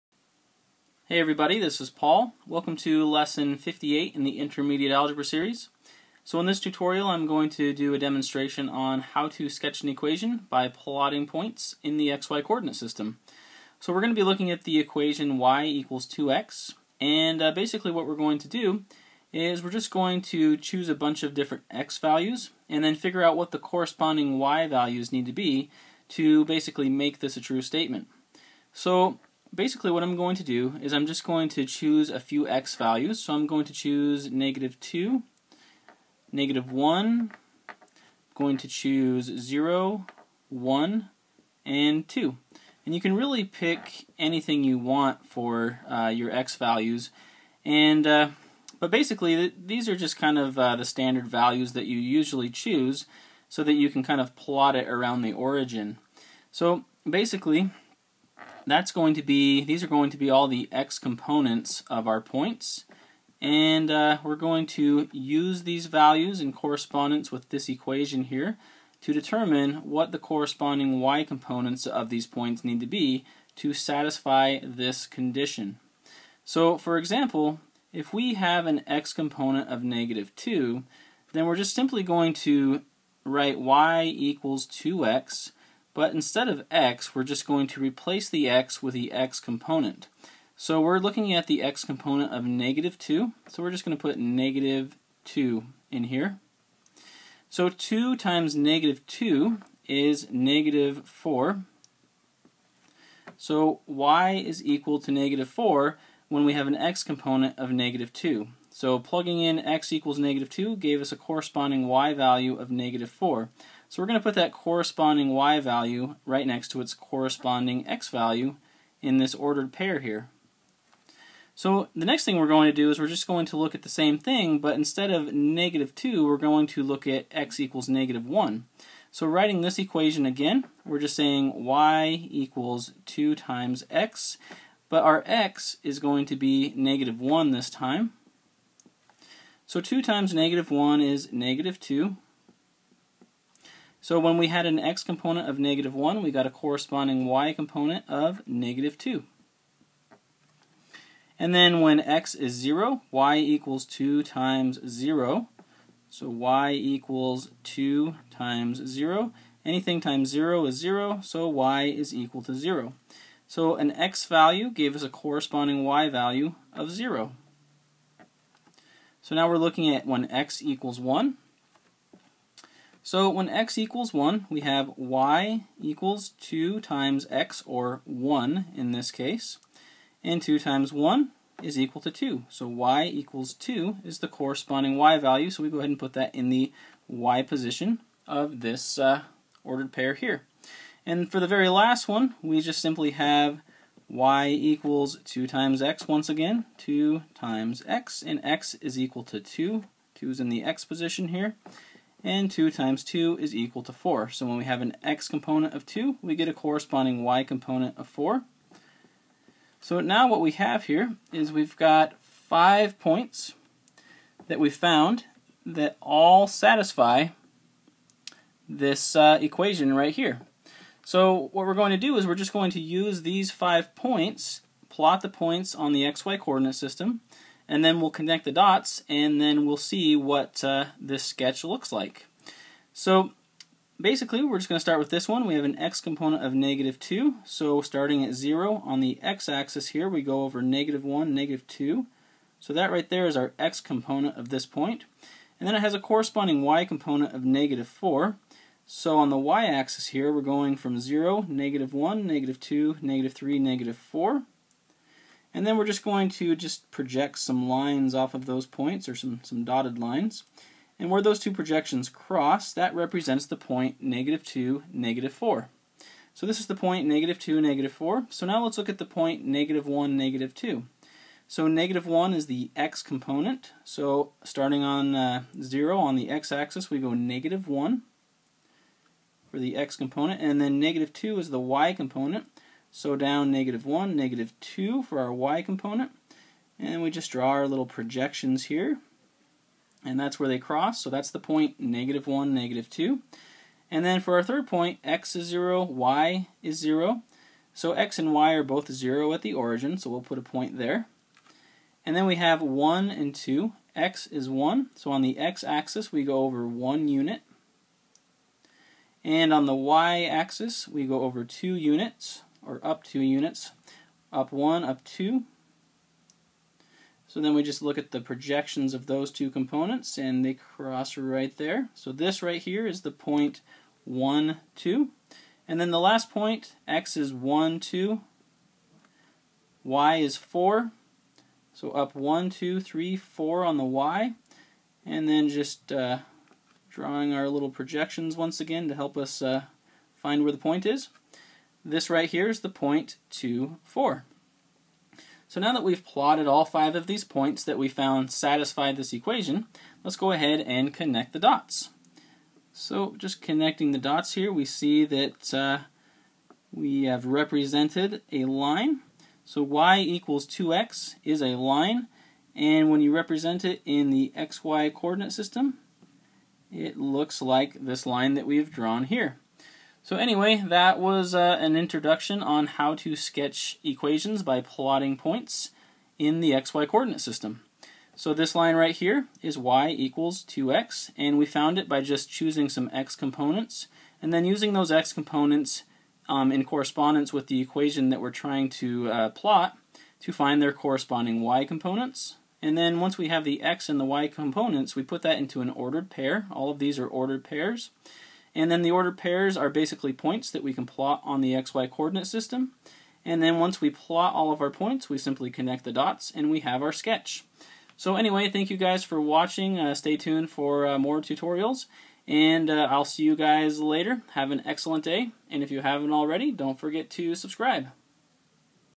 How to Sketch an Algebraic Equation by Plotting Points, Intermediate Algebra , Lesson 58
In this tutorial I demonstrate sketch the equation y = 2x by plotting points in the X Y coordinate system.